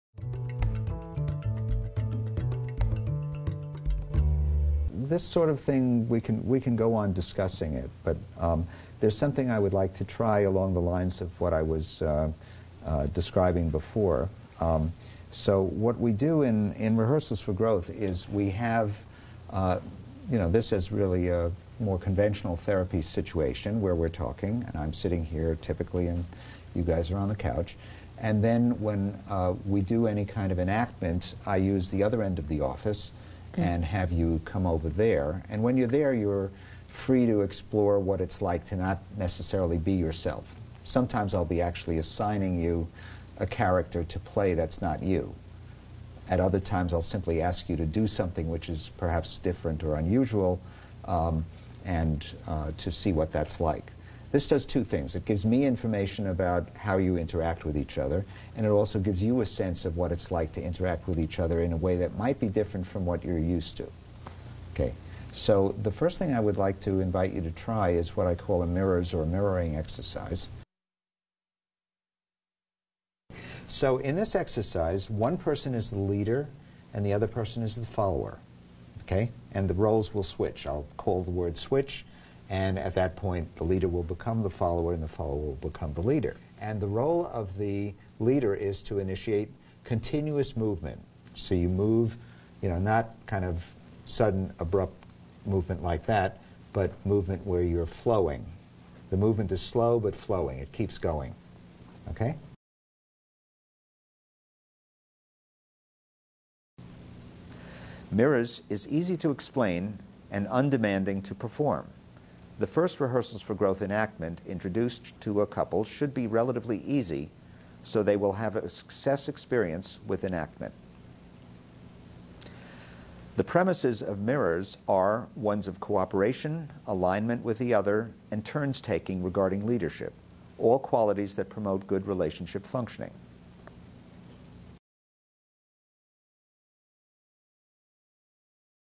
Using Drama Therapy with Couples Video
Dr. Daniel Wiener demonstrates how to energize and enhance your couples work by incorporating drama therapy techniques called Rehearsals for Growth.